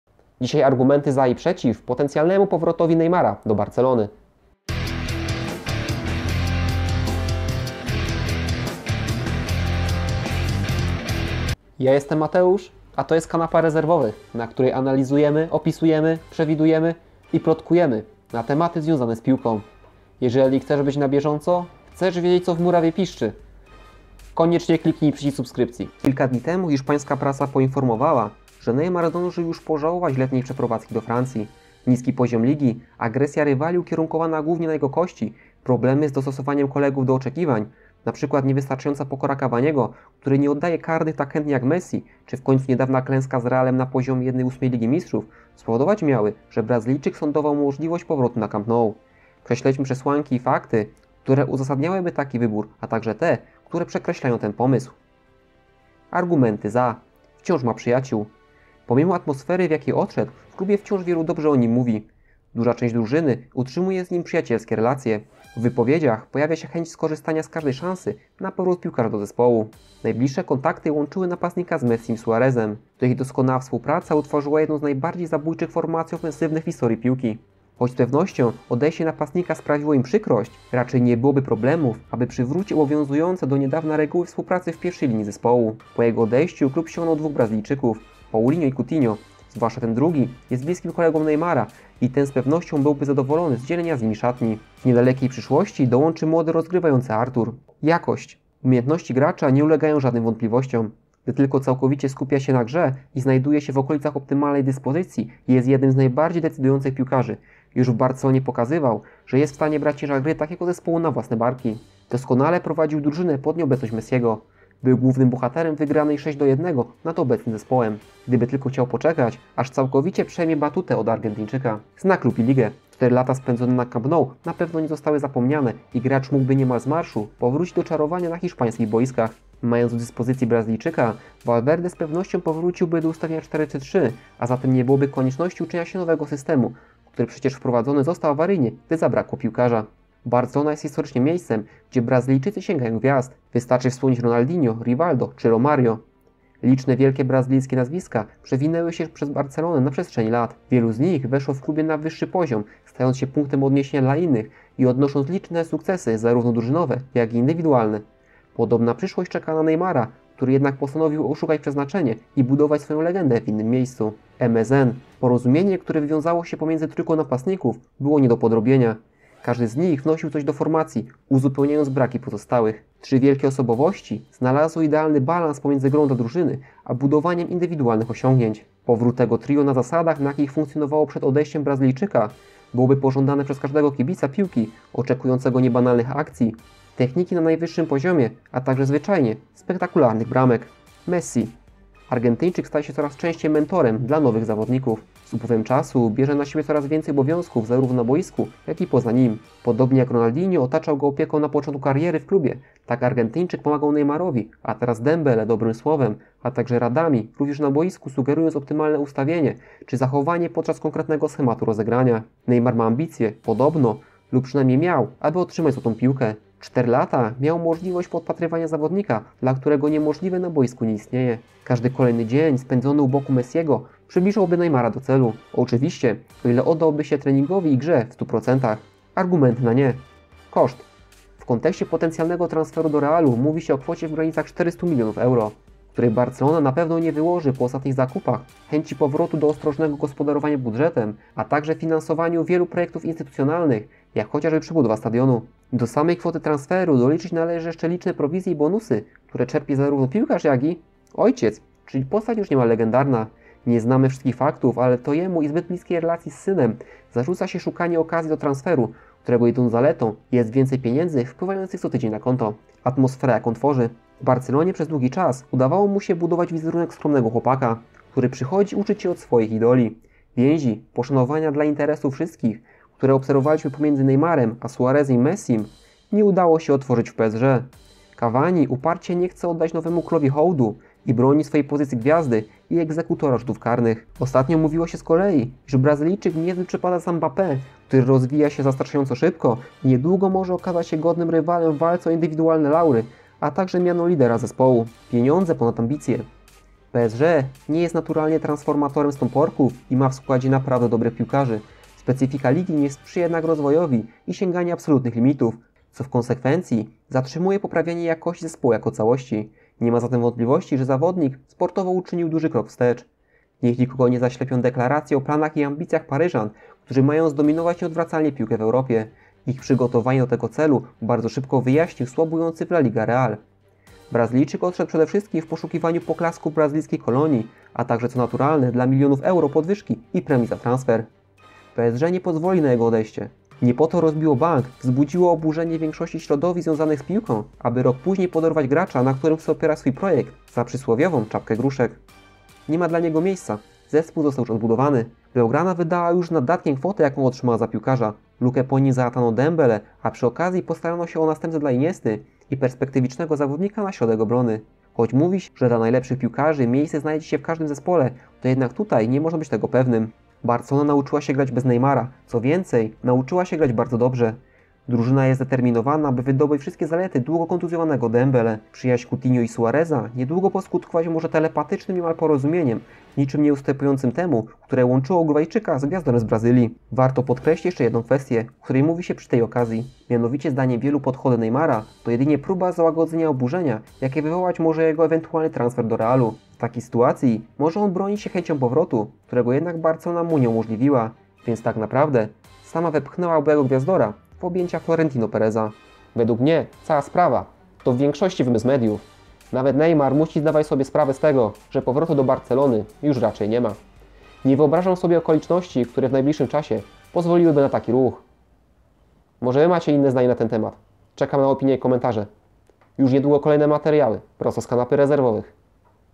Powrót Neymara - dlaczego tak, dlaczego nie
Sprawdz koniecznie: "Transfery Atletico Madryt 2018"
Analiza sytuacji brazylijskiego napastnika PSG, Neymara w kontekście potencjalnego powrotu do byłego klubu - Barcelony, lub też transferu do Realu Madryt.
Jakie przesłanki pozwalają myśleć, że Naymar mógłby znów grać na Camp Nou razem z Messim, a jakie kategorycznie wykluczają taką możliwość.

Infografiki:
Banner vector created by Freepik
Infographic wektory designed by Freepik